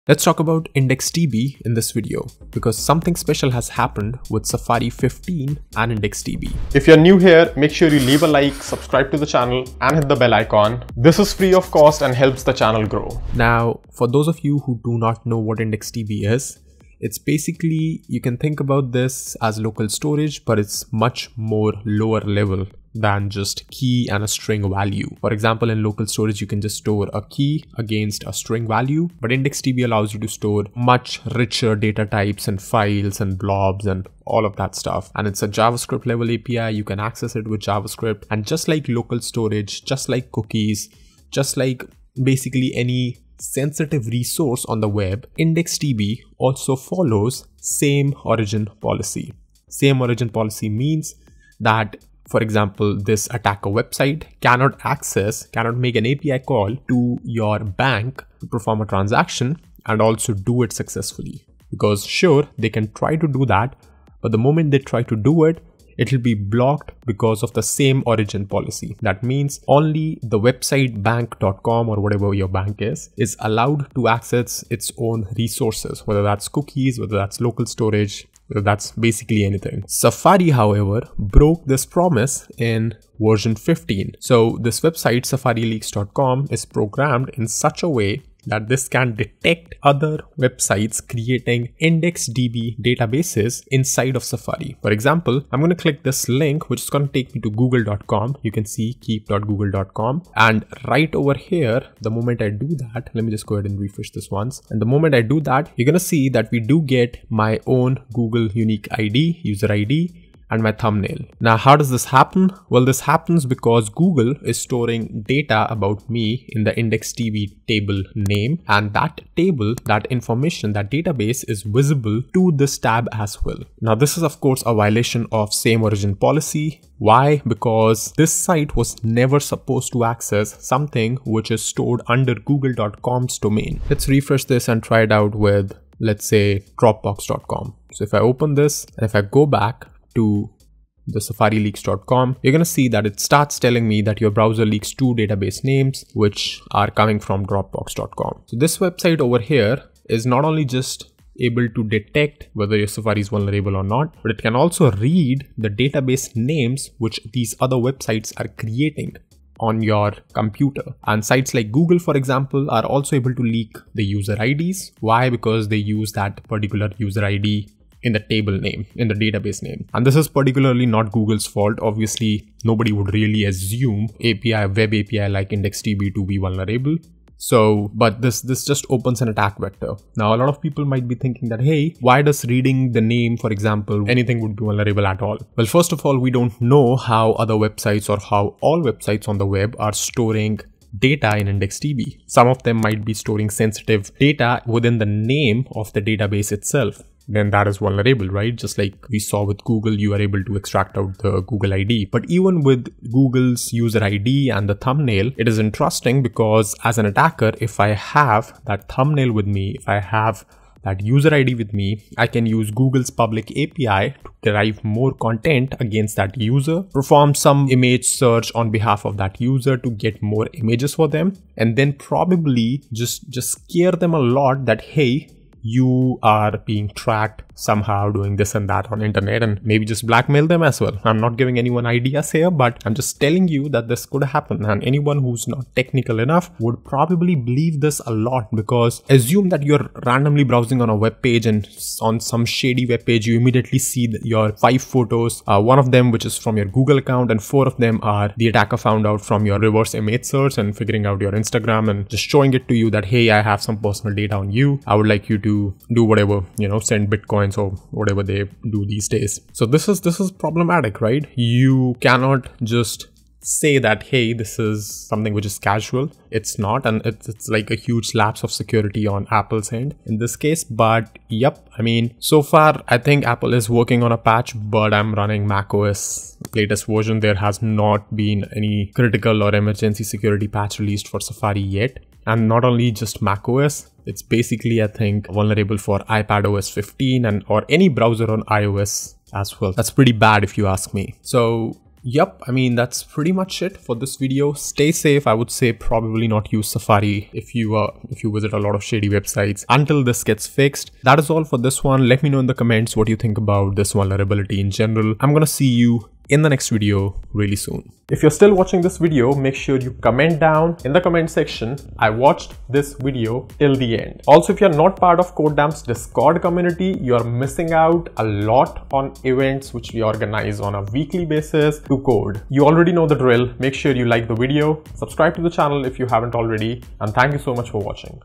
Safari is leaking your private info, are you using it?
IndexedDB is a browser API for client-side storage designed to hold significant amounts of data. In this video, let's talk about Safari 15 IndexedDB and the vulnerability affecting the Safari browser which can leak a user’s identity and their website history.

Timestamps
0:00 Teaser
0:13 IndexedDB
0:50 Same Origin Policy
1:21 Vulnerability in Safari 15
4:30 Conclusion
5:08 Outro

✨ Video Tags ✨
Vulnerability in Safar 15
Same-origin violation Vulnerability in Safari 15 could leak a user’s website history and identity